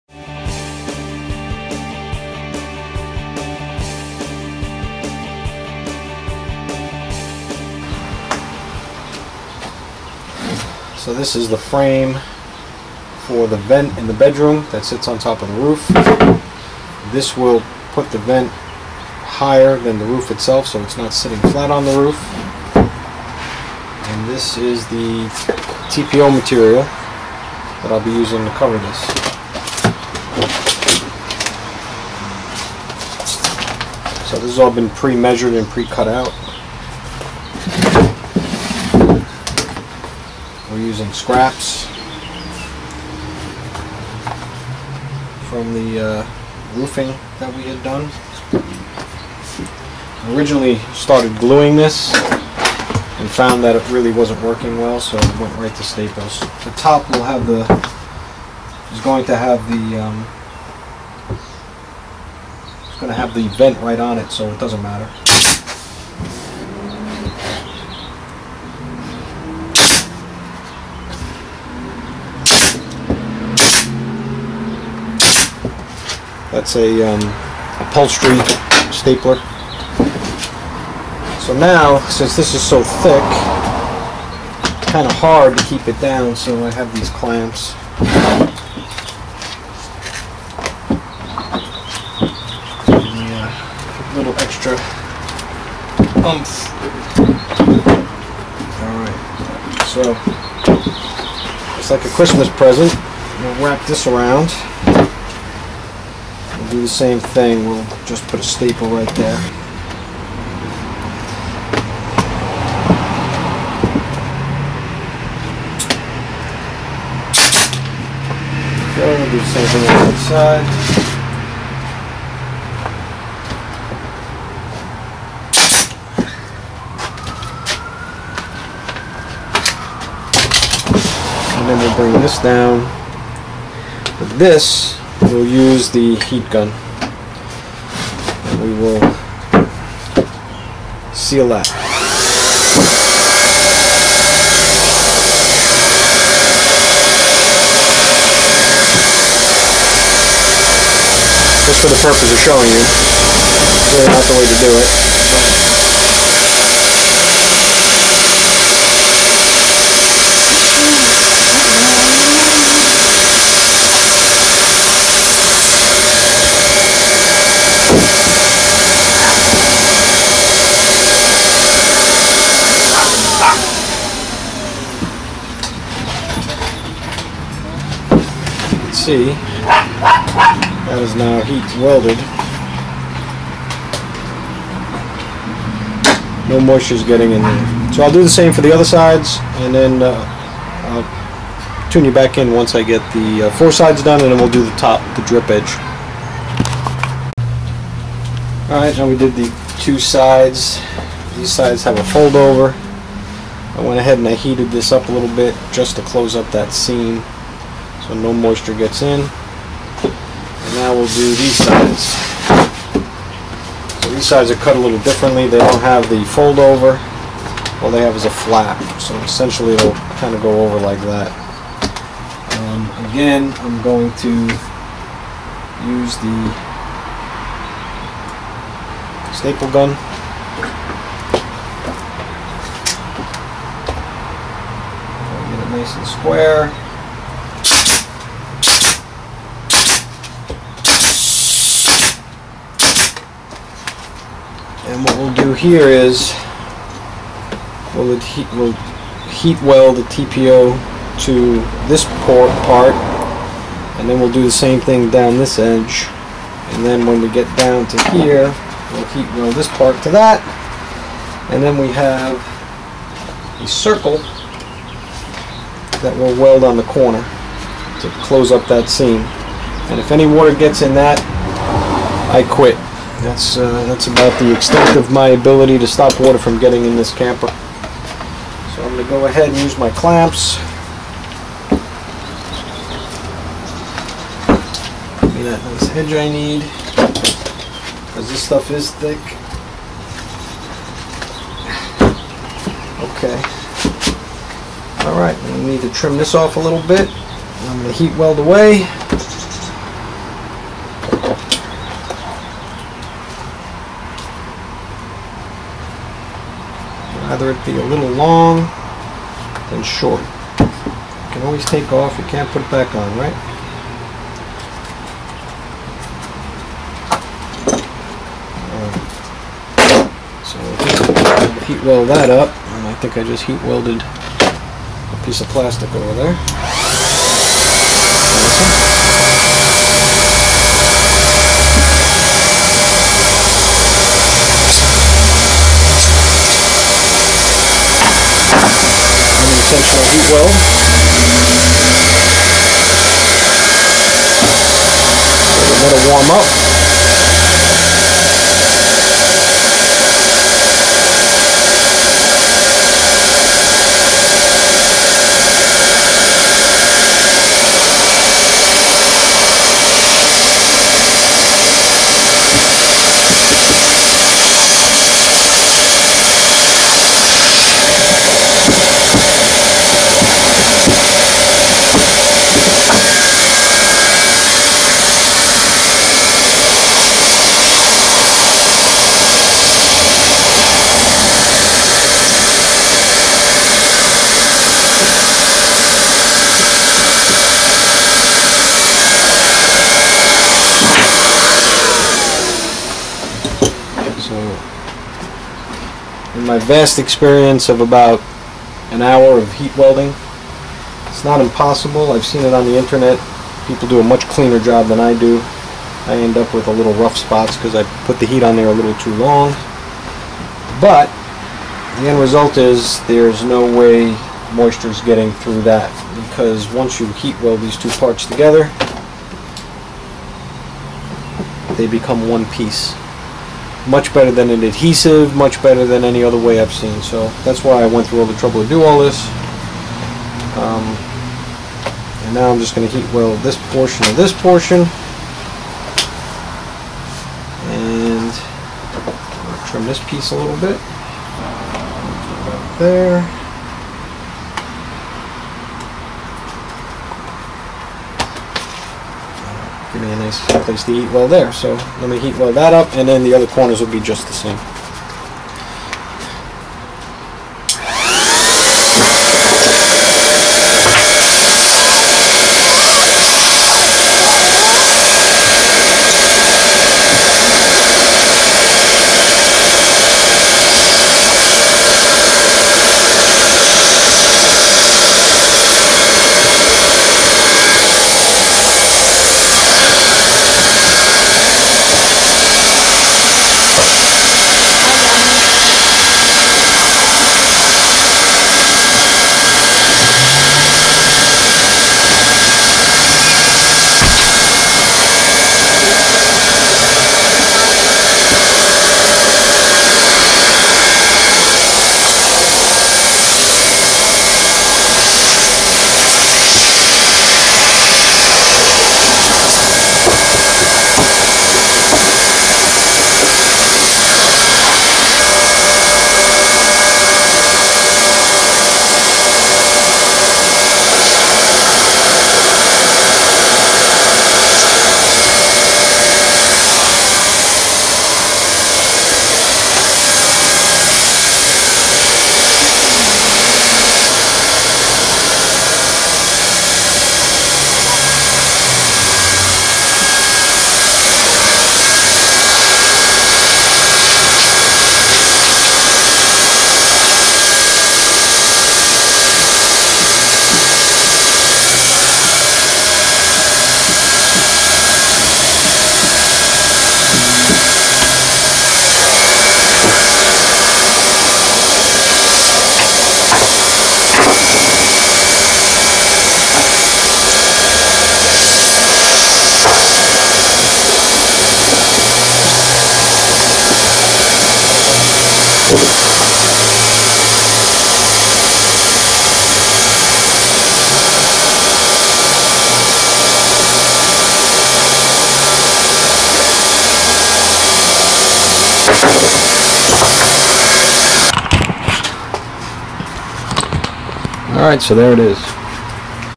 RV Roof Repair Part 5
Replacing roof on 2004 Keystone Springdale - demonstrating how to fabricate curbs and heat weld TPO for vents, antennas, and ac's on Travel Trailer roof. Third in a series.